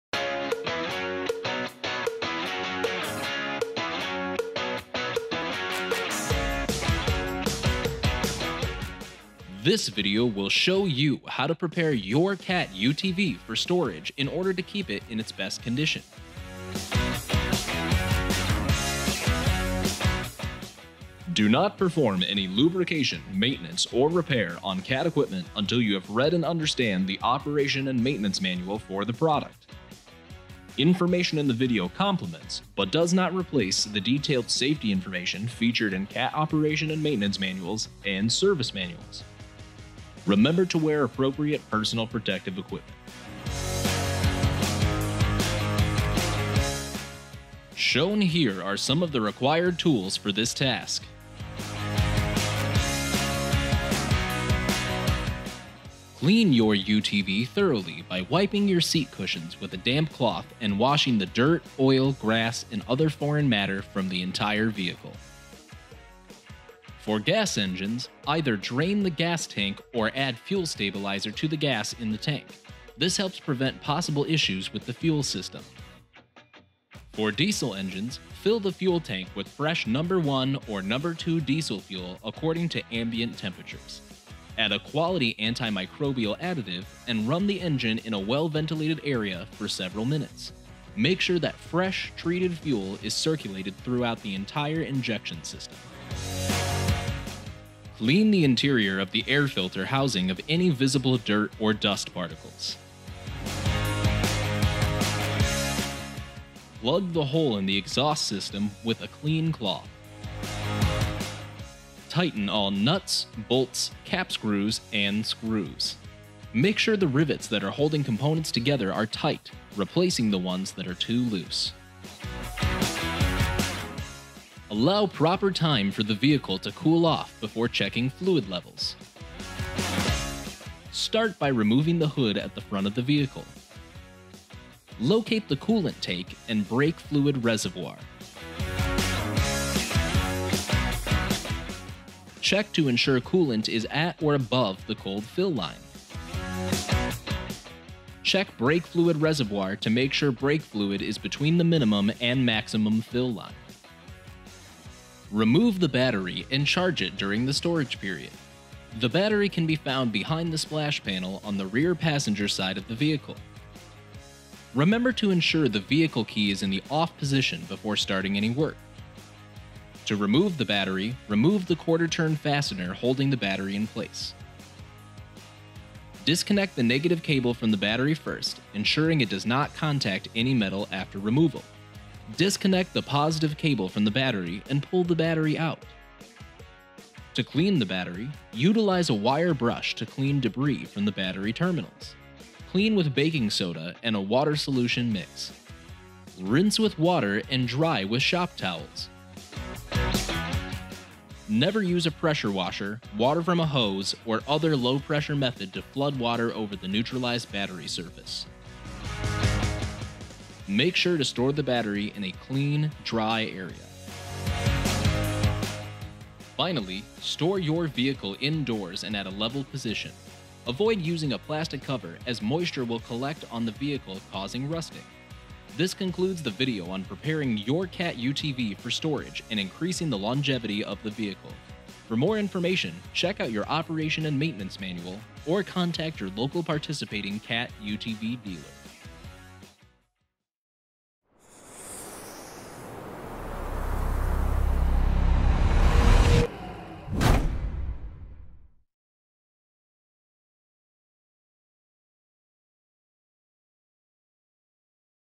Prepare Cat® Utility Vehicles for Storage | CUV82, CUV85, CUV102 D, CUV105 D
This video guides you through how to prepare your Cat Utility Vehicle for storage.